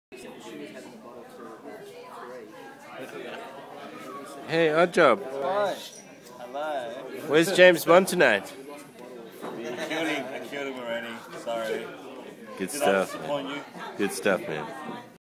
Interview With Odd Job
Odd Job speaks out about his frustrations with bond